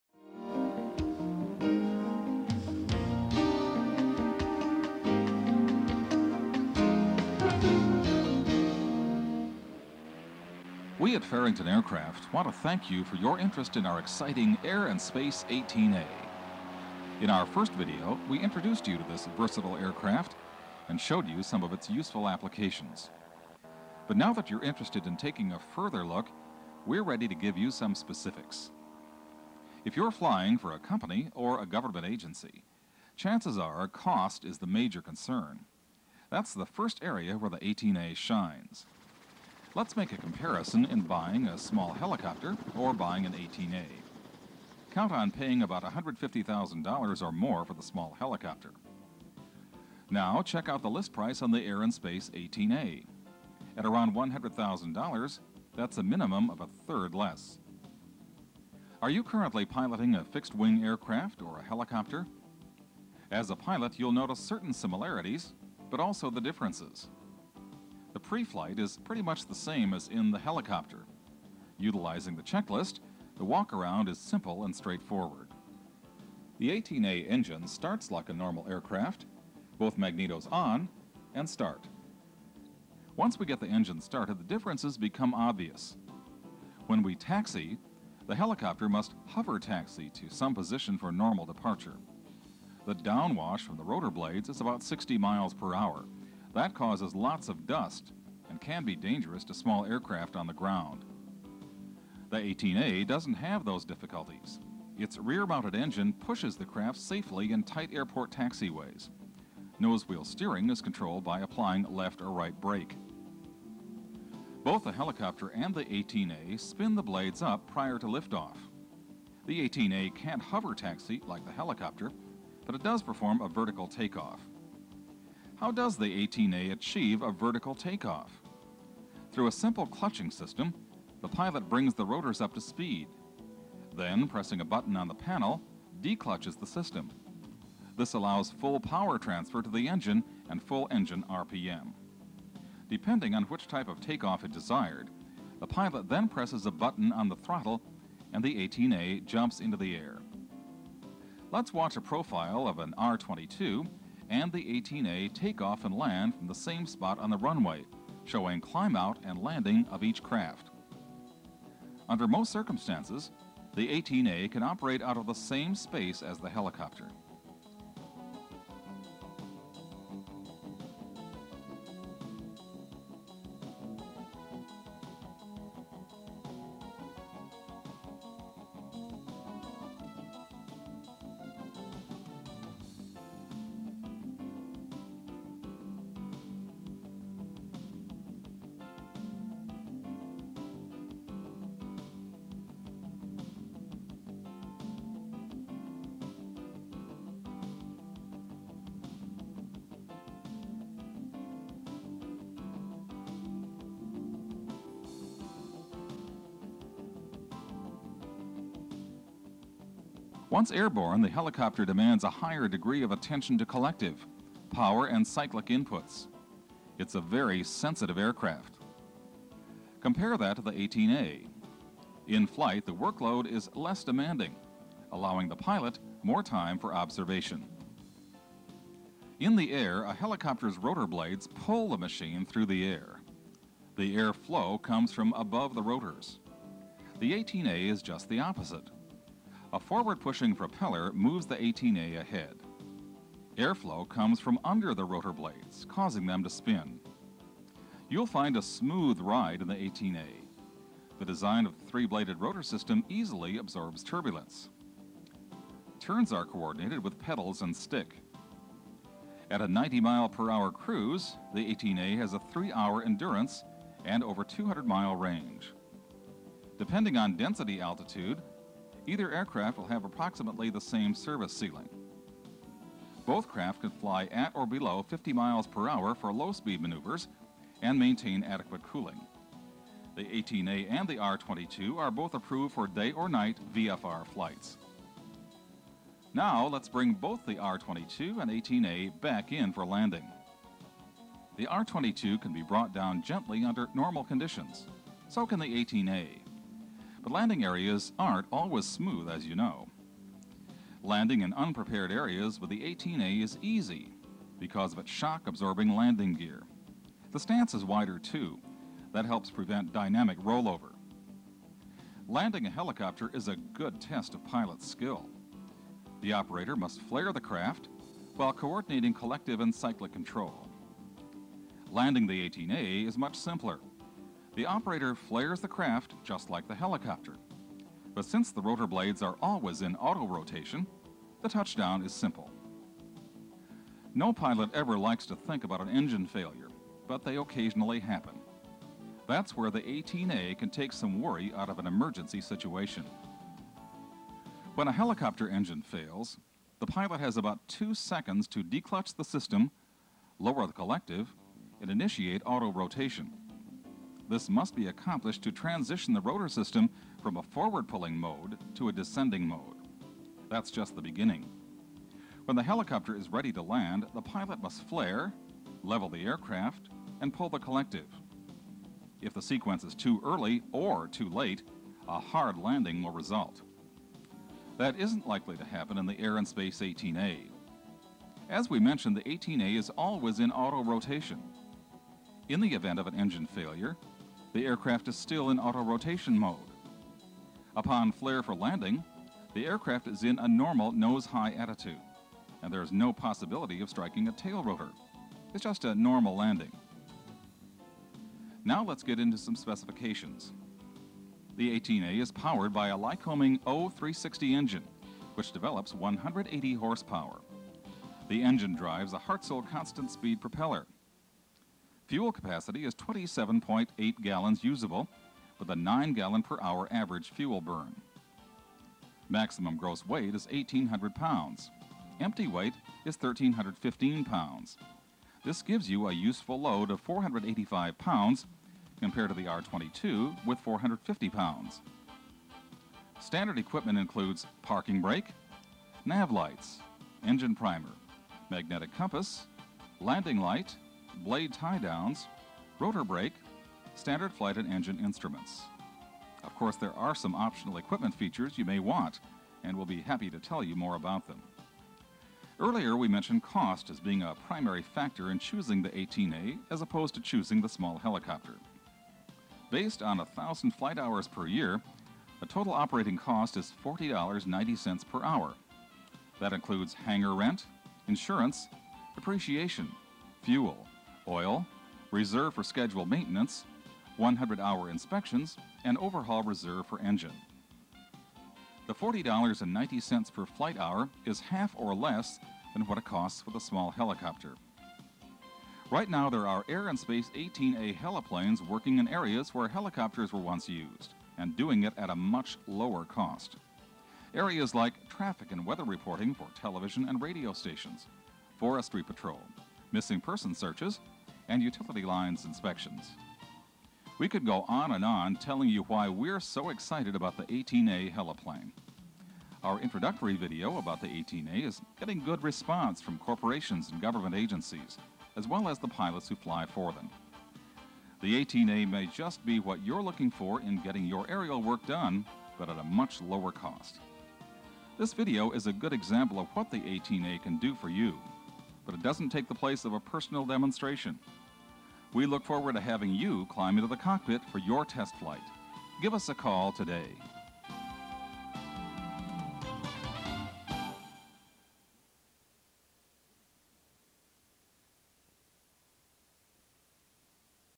18A Pt2
Review of the Air & Space 18A Gyroplane (1993) Part 2 includes comparison with R22 helicopter.